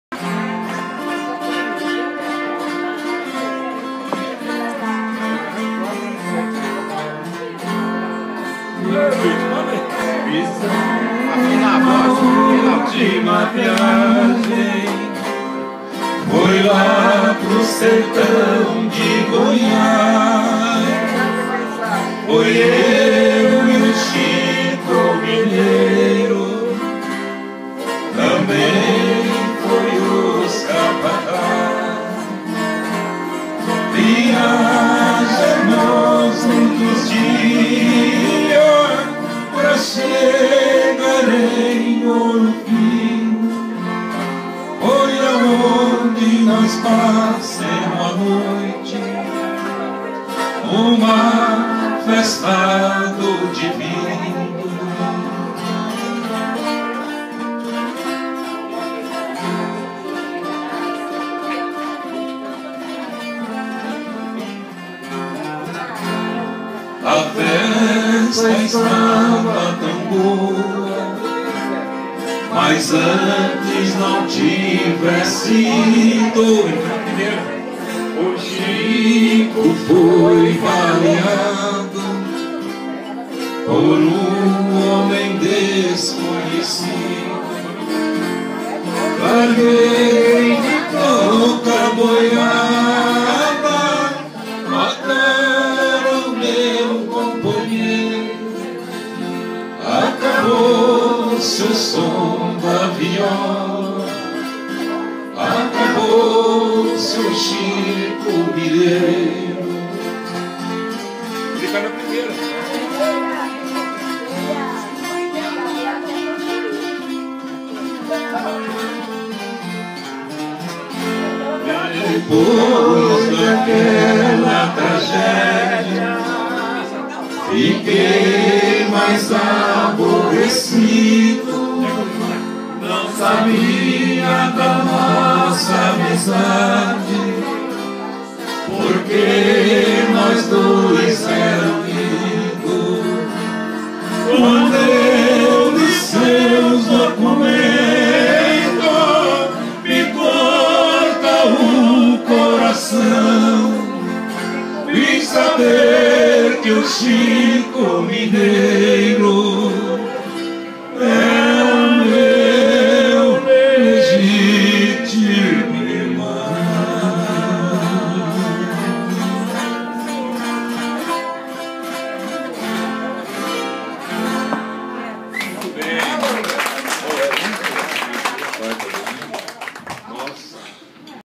Primeiro sarau cantina Piatto com José Romeu e Noel Rosa.
Foi a primeira vez que cantamos juntos, foi um "chora viola" muito bom..